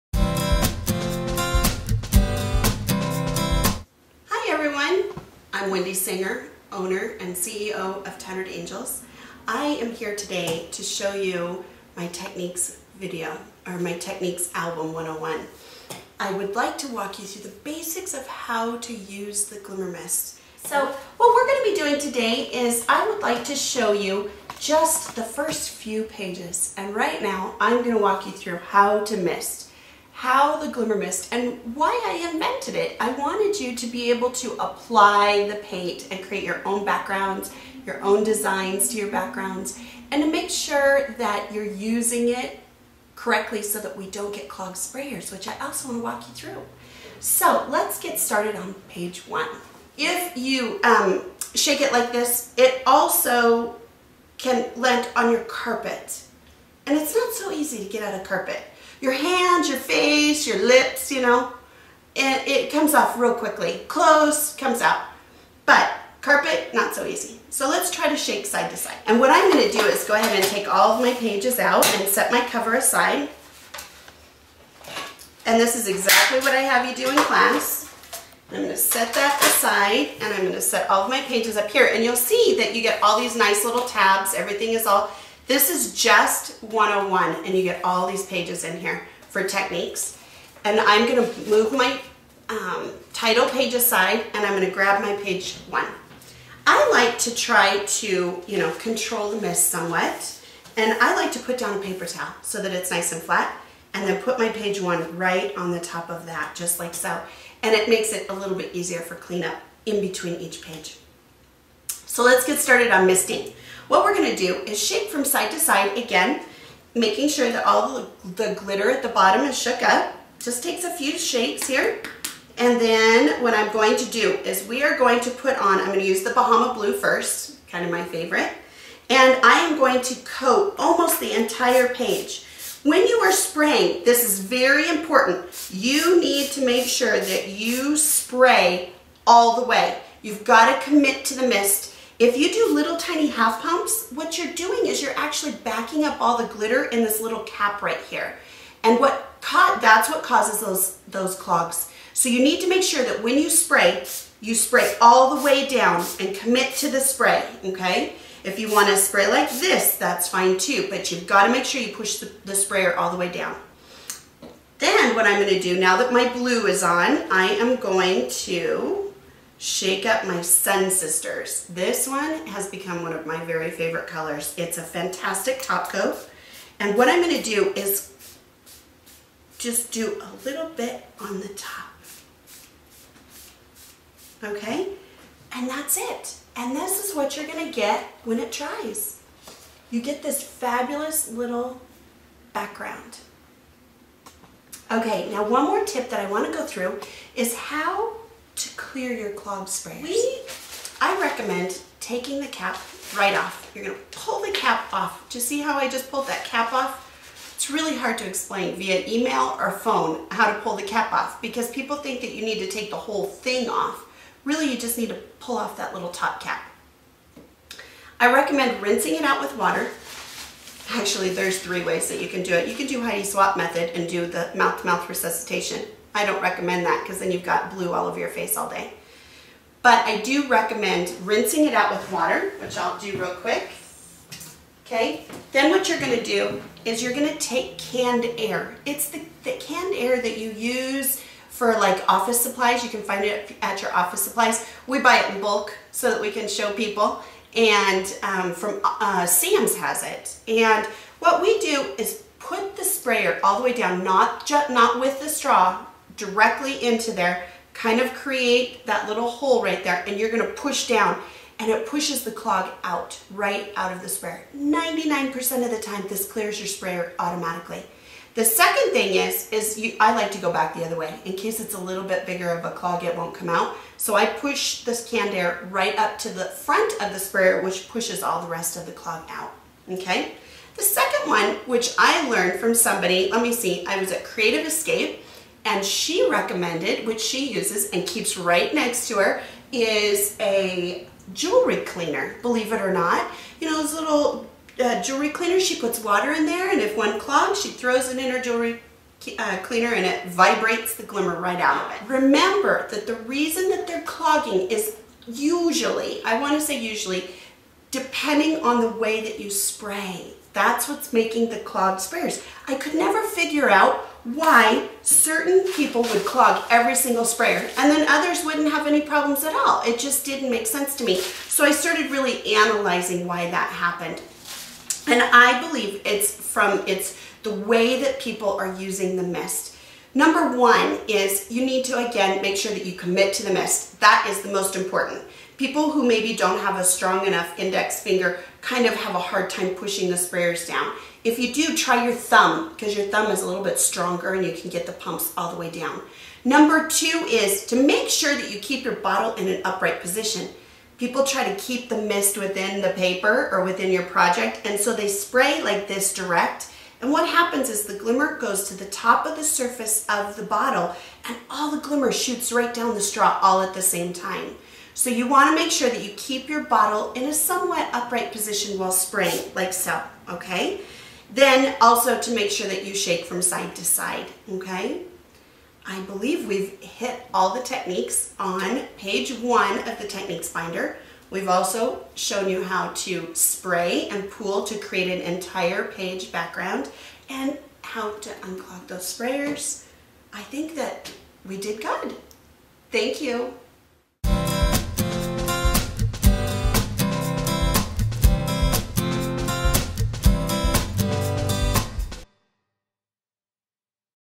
Blending GM and Clogged Sprayers.m4v
Learn how to blend different colors of Glimmer Mist to make your own colorized creations and how to unclog your Glimmer Mist sprayers.